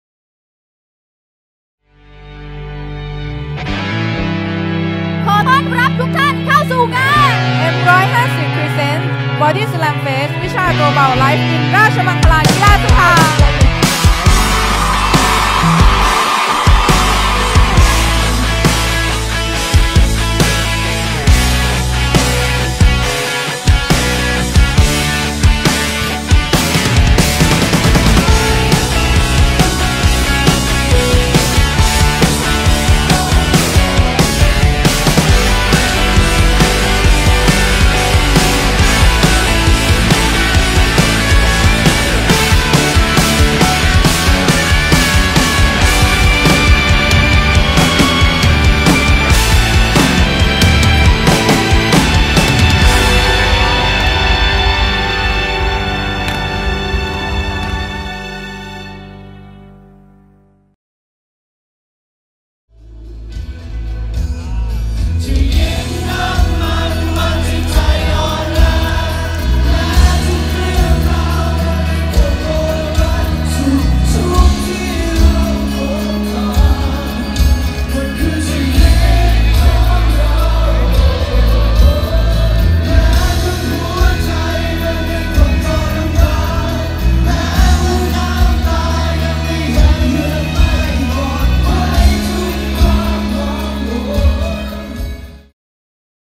คอนเสิร์ตแรกของเรา BODYSLAMFEST
ในมุมมองของเรา  จุดเล็กๆ ท่ามกลางคนหลายหมื่นคน  โคตรมีคความสุข โคตรเหนื่อยบ โคตรประทับใจเลยที่ได้ไปอยู่ตรงนั้น